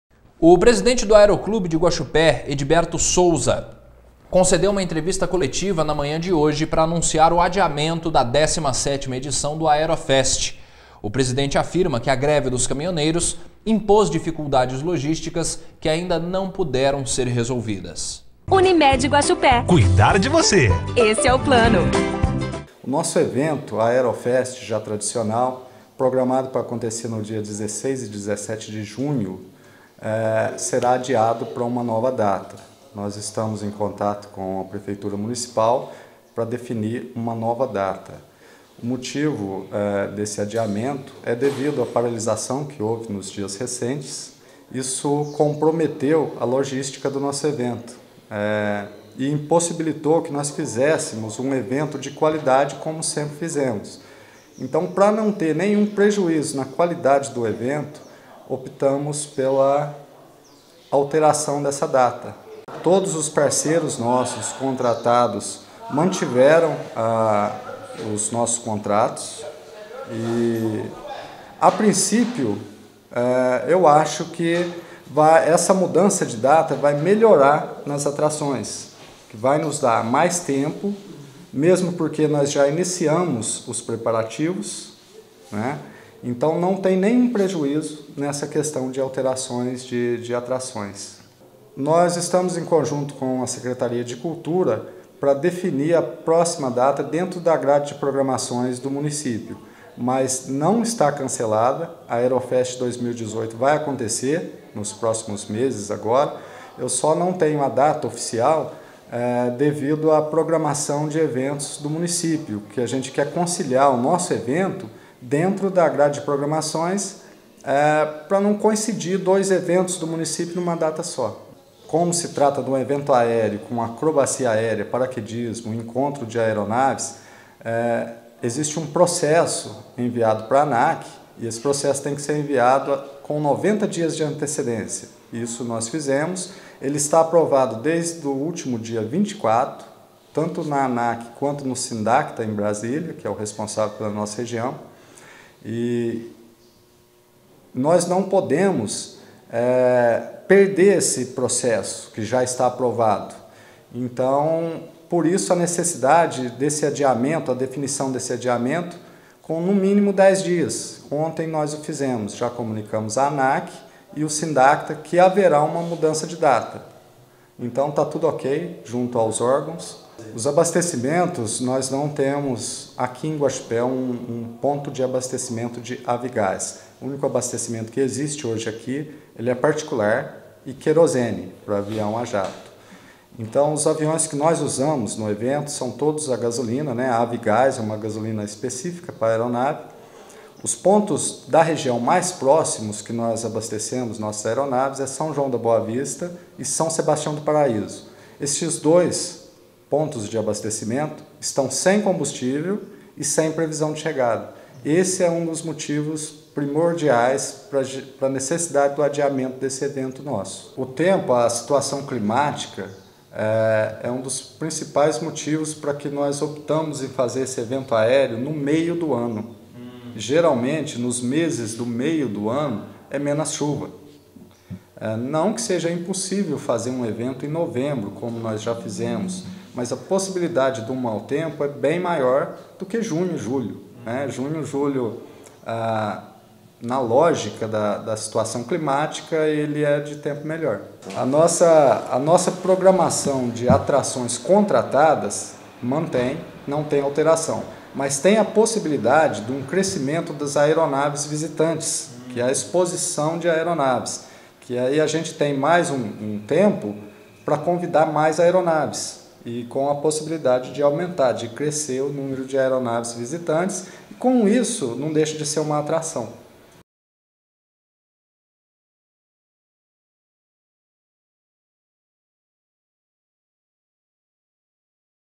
Aeroclube adia Aerofest por problemas de logística em razão da greve dos caminhoneiros
O presidente do aeroclube de Guaxupé, Ediberto Souza concedeu entrevista coletiva nesta manhã para anunciar o adiamento da 17ª edição do Aerofest. O presidente afirma que a greve dos caminhoneiros impôs dificuldades logísticas que ainda não puderam ser resolvidas.